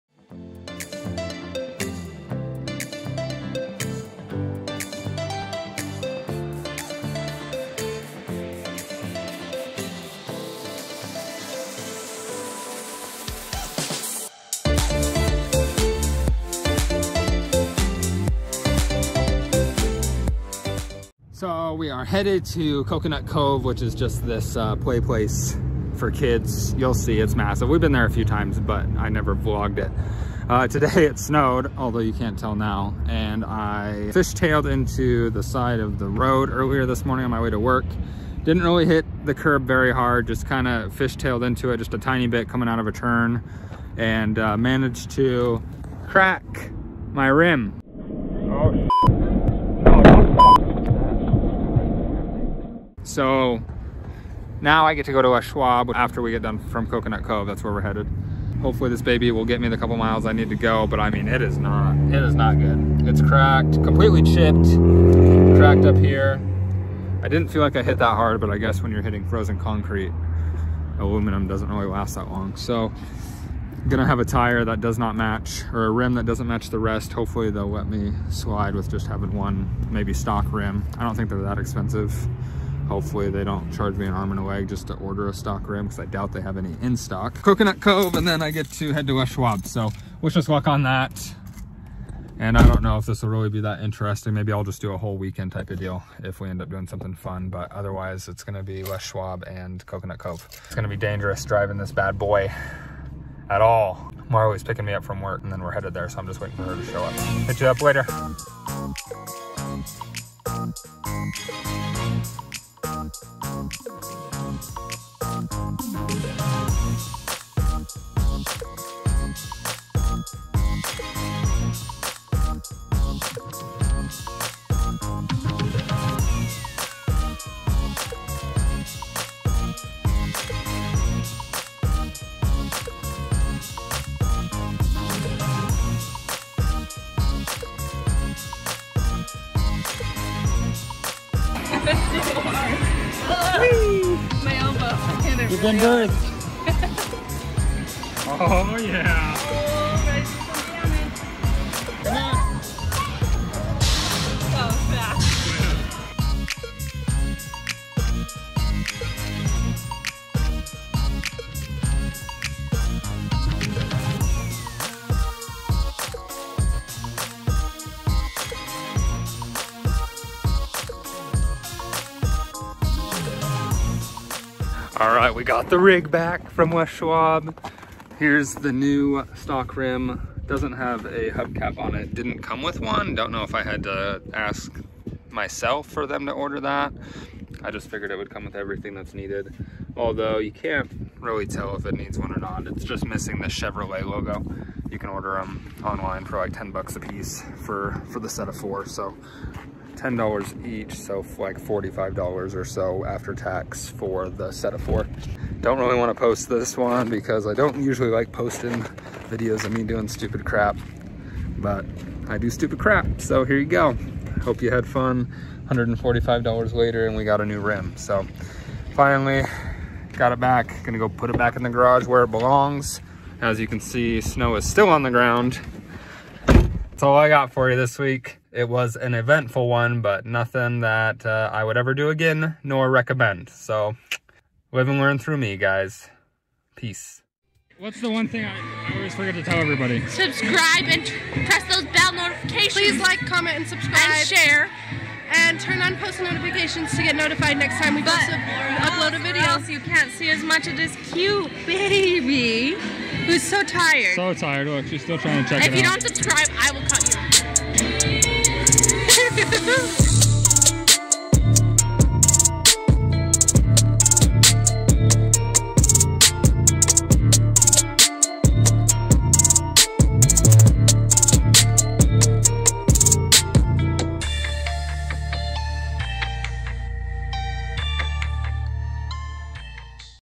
Daughter Had SO Much Fun at This Play Place in Utah Valley
This week was short and sweet, but definitely includes some footage you do NOT want to miss!

This video was shot with a Canon M50 with a RODE shotgun Mic along with the 15 - 45 mm stock lens and an iPhone 11 Pro Max